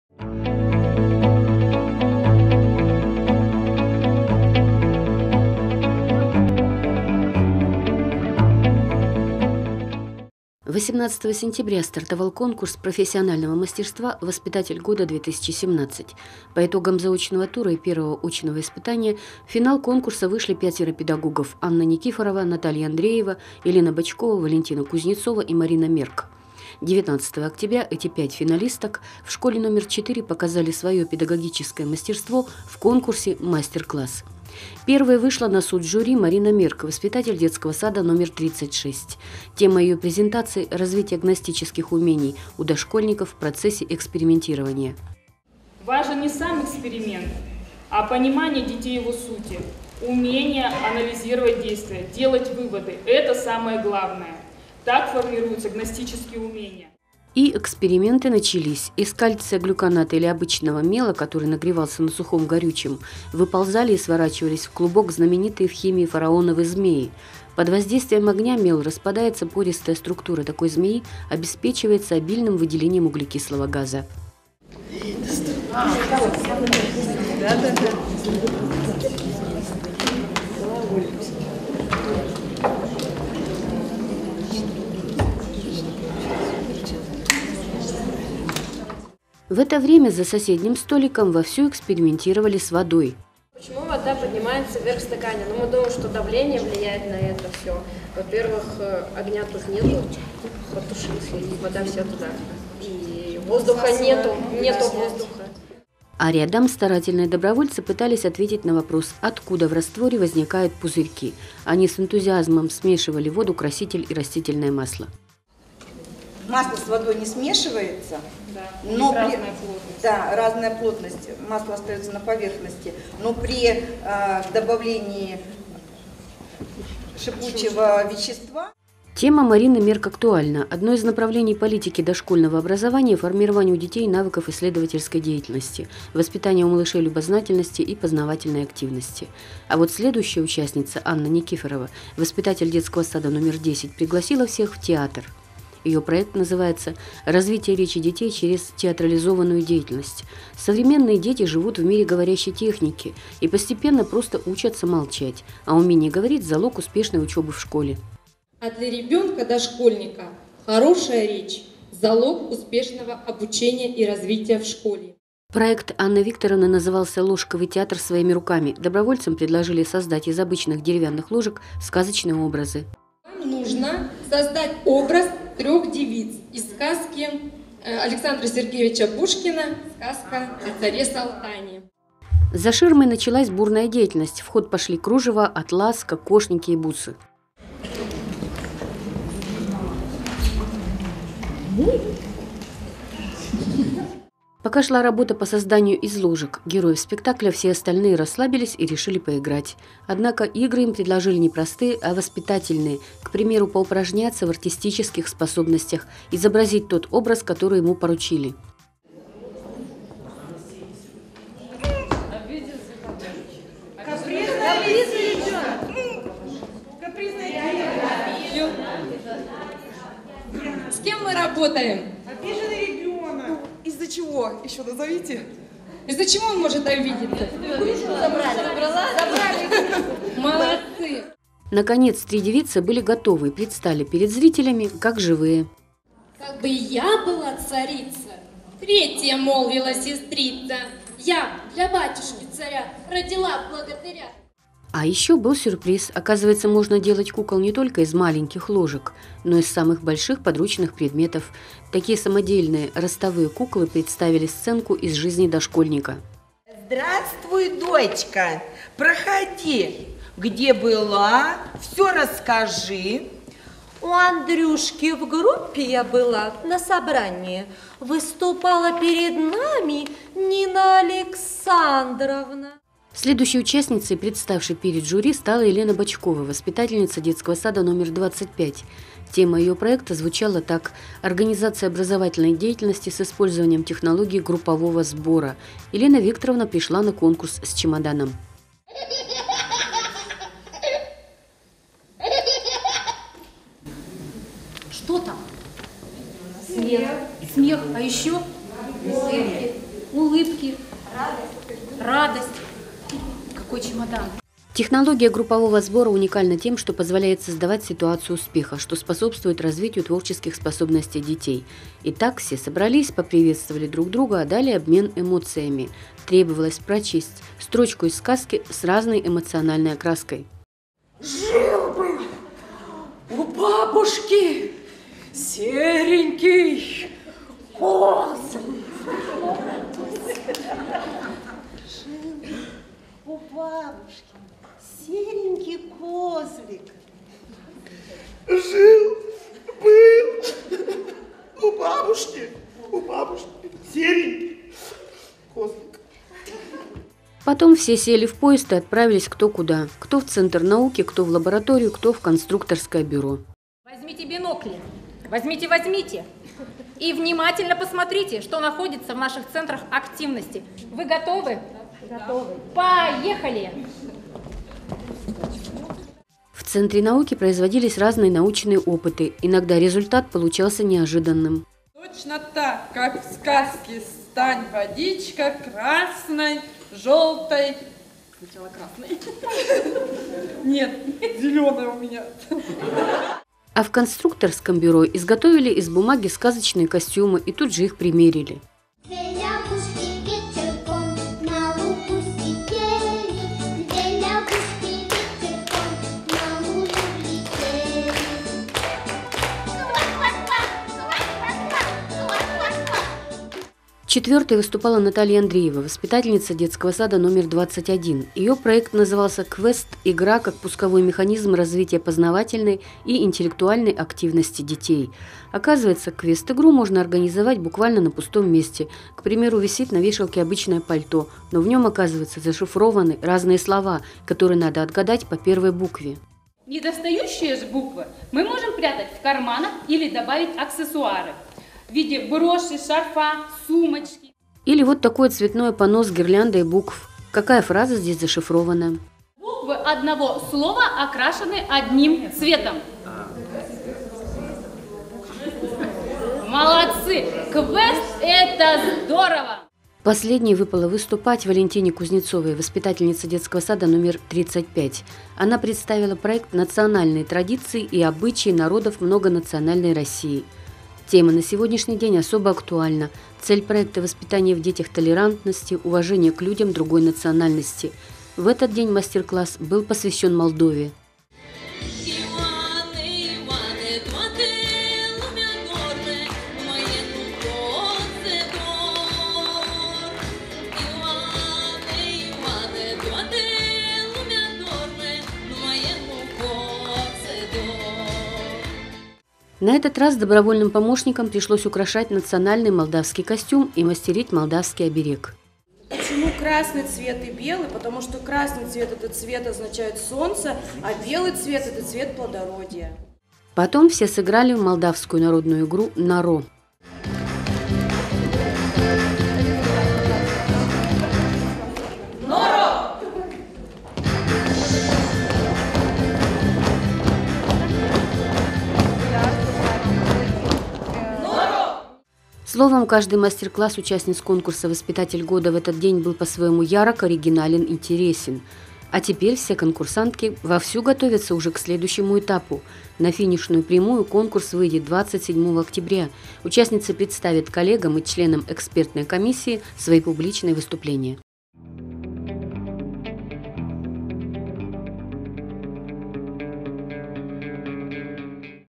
Мастер-классы участниц конкурса «Воспитатель года - 2017»
По итогам заочного тура и первого очного испытания в финал конкурса  «Воспитатель года - 2017» вышли пятеро педагогов:  Анна Никифорова, Наталья Андреева , Елена Бочкова,  Валентина Кузнецова и Марина Мерк.  19 октября  пять финалисток в школе № 4  показали свое педагогическое мастерство в конкурсе «Мастер-класс».